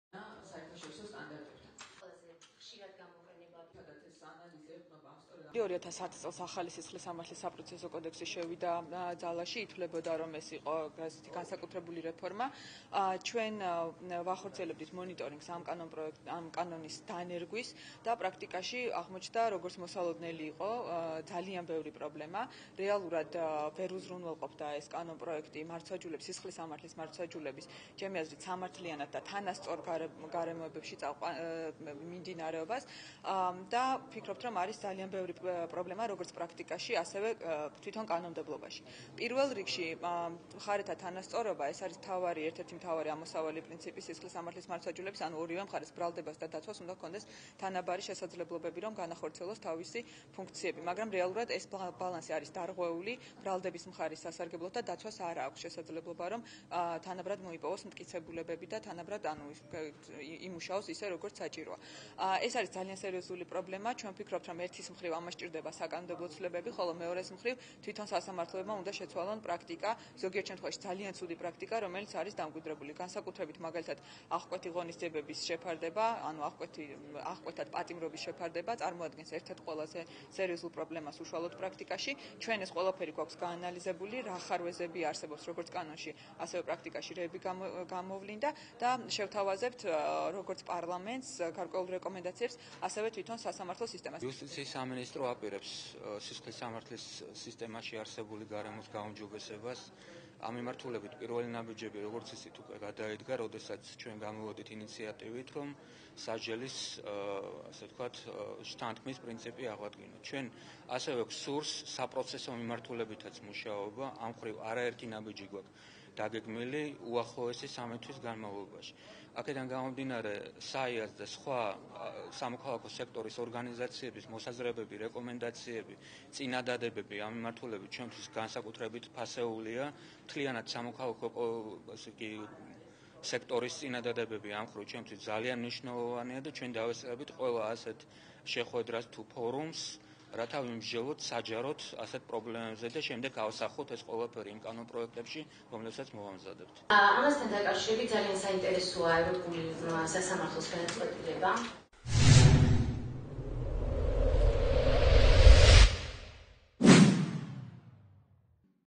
ახალგაზრდა იურისტთა ასოციაცია
„სამოქალაქო სექტორის შემოთავაზებები და რეკომენდაციები ჩვენთვის მნიშვნელოვანია," - სანდრო ბარამიძე.